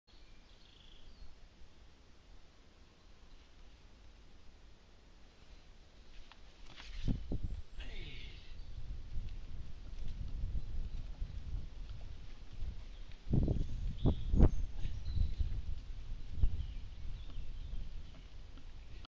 Ben Dyno Variation 7A
A dyno eliminate of the climb Ben on the Hargreaves Buttress. A leap from the obvious starting break to the top.